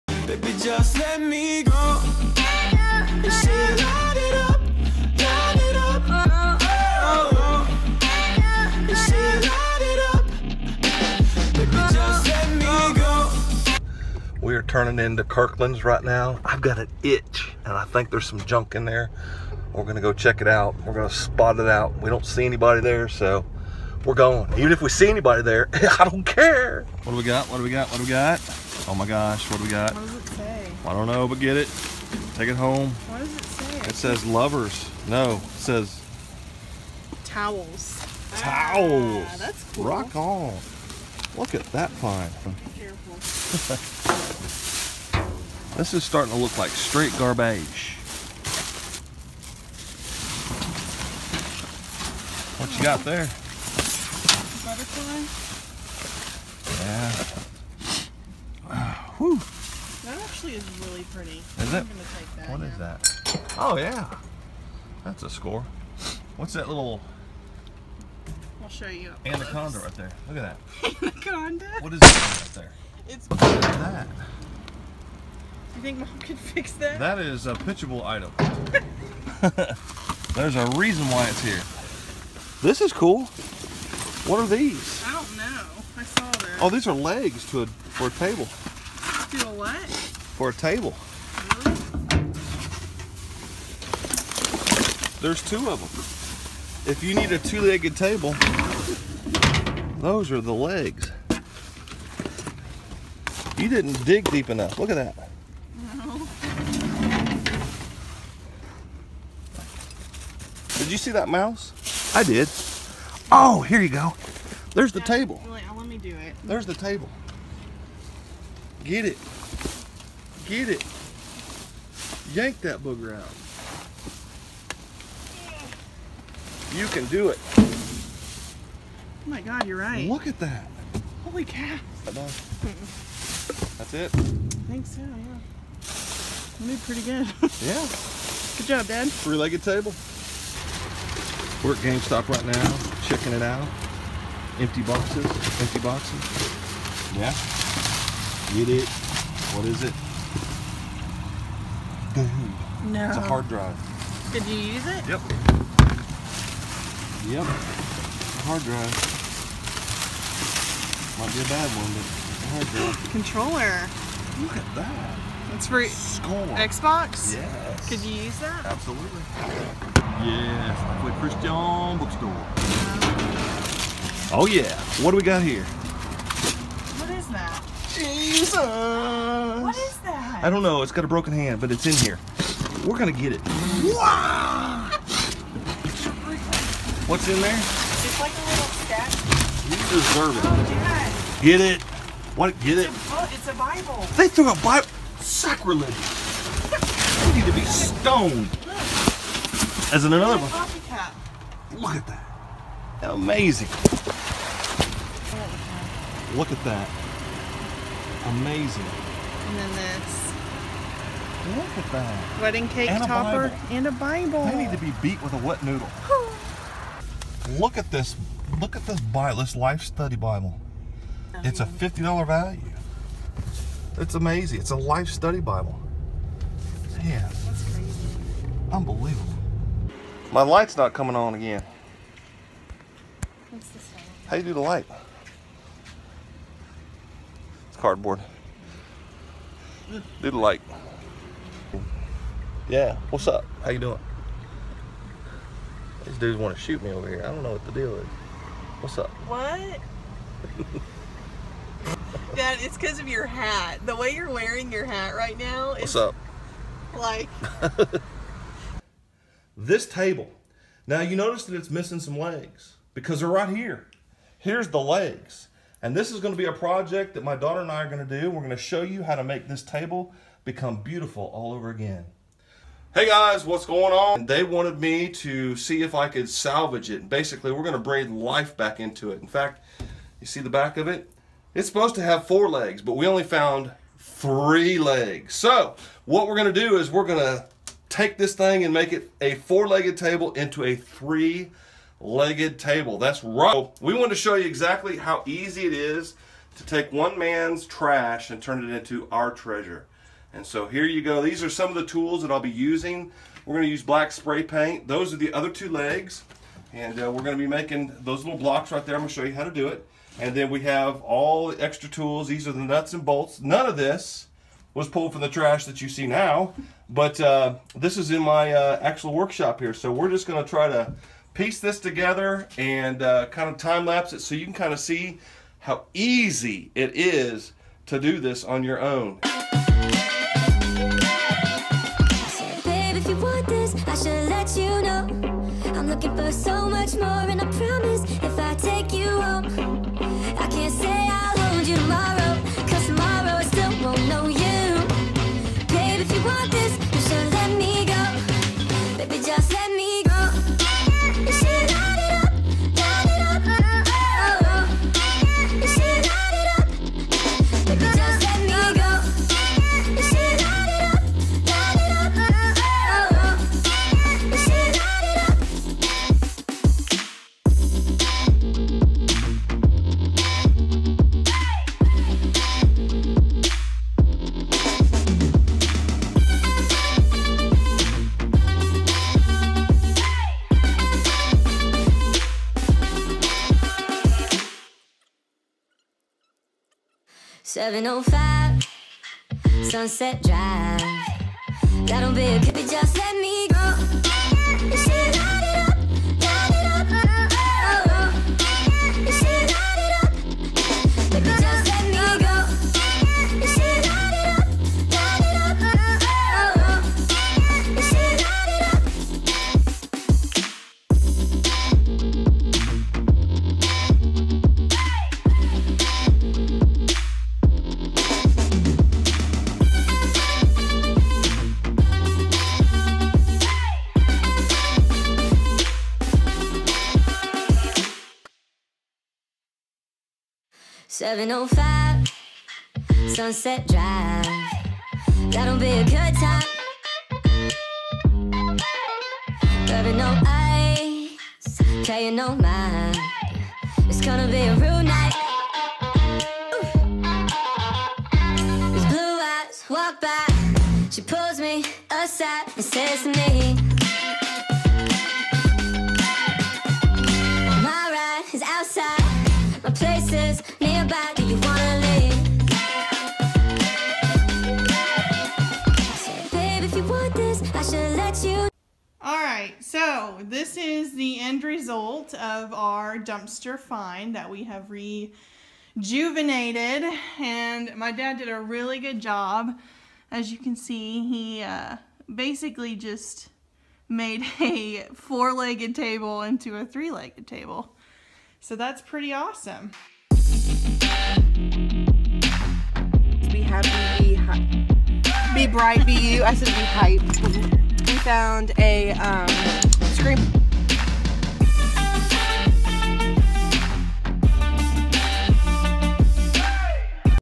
KIRKLANDS DUMPSTER || DIY PROJECT 🛠
You guys asked for him... HE'S BACK!

Watch as we venture out with our dad on an extreme dive while we restore a broken piece of furniture as my dad walks us through it!